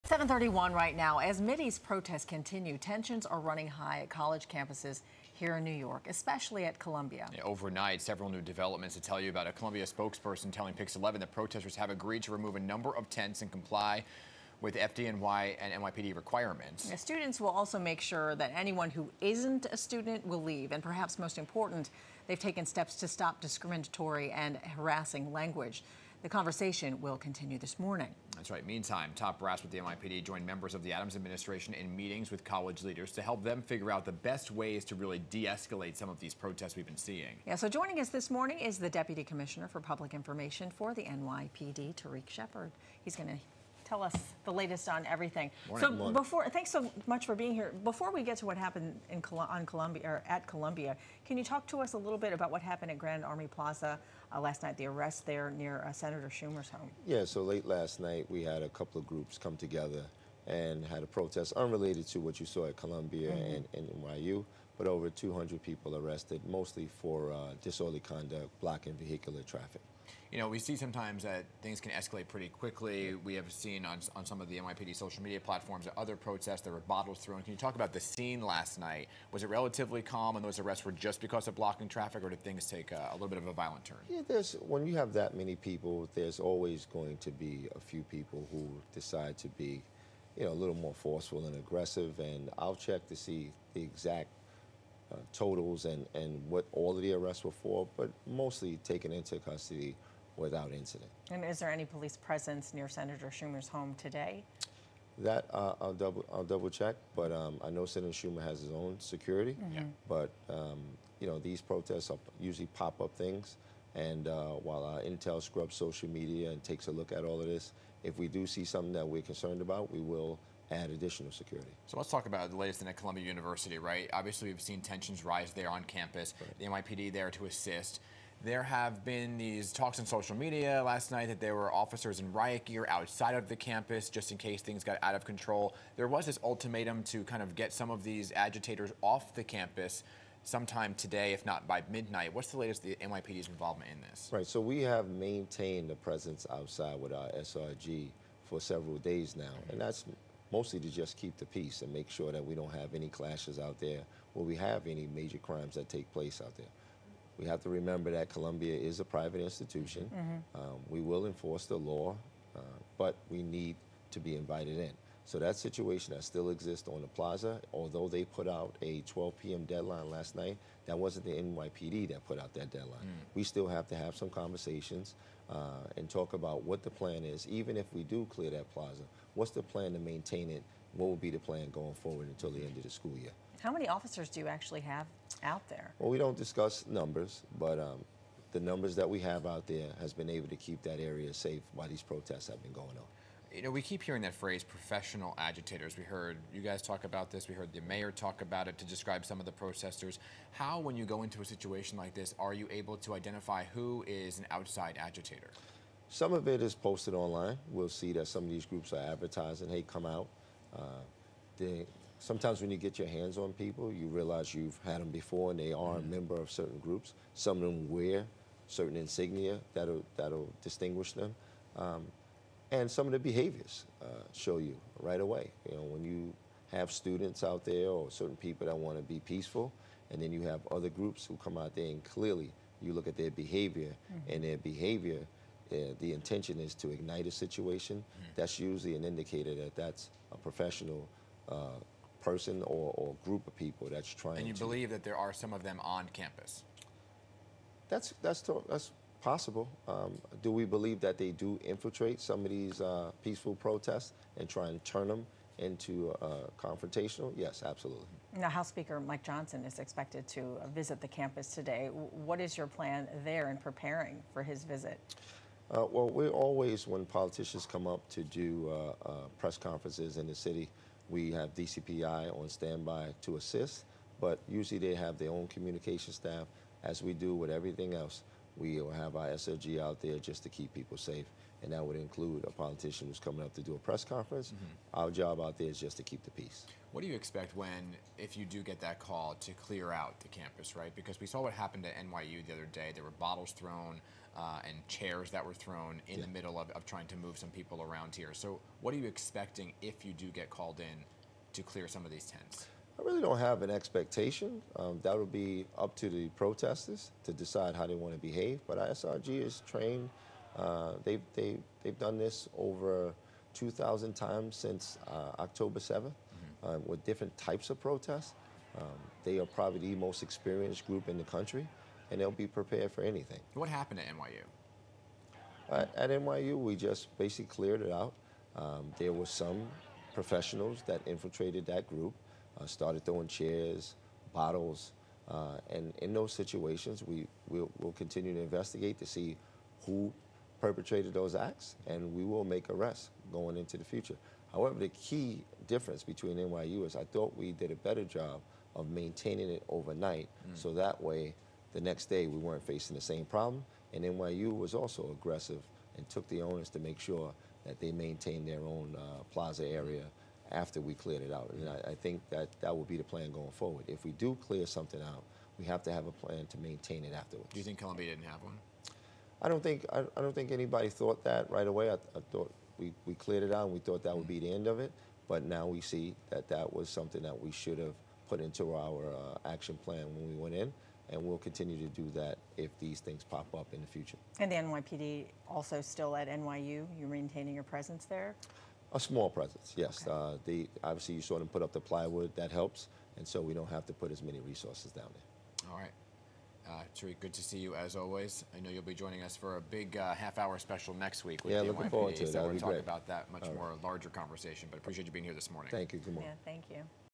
NYPD commissioner on de-escalating protests
Tensions are running high at college campuses in New York and across the U.S. as pro-Palestinian protests continue.